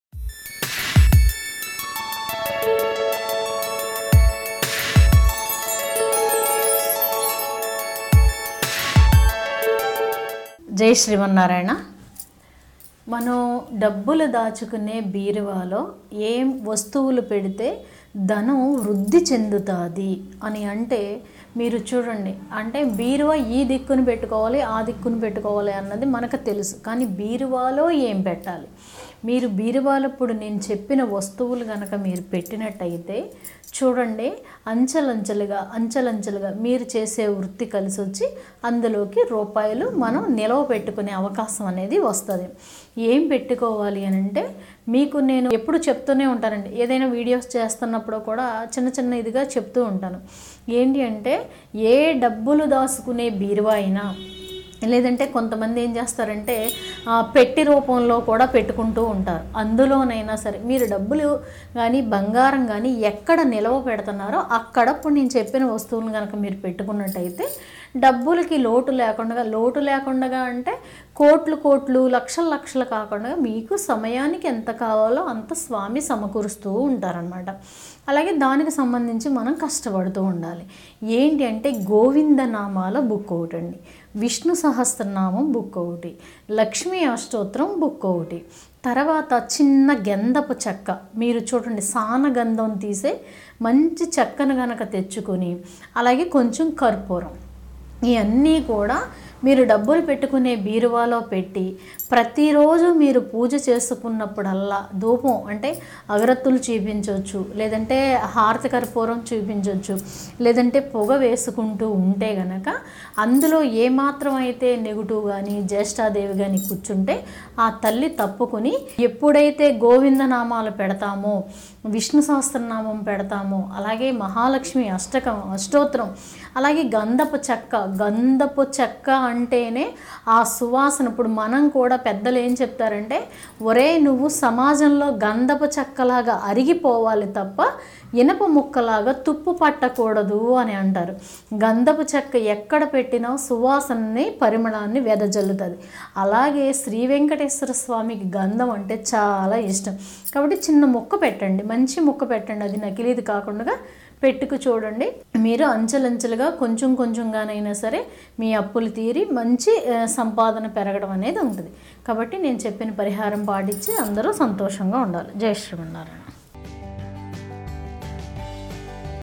బీరువాలో ఈ వస్తువులు పెడితే ధనానికిలోటుండదు - Goddess Lakshmi Devi Telugu - Dhana Yoga In Telugu
Astro - Numerologist
Elur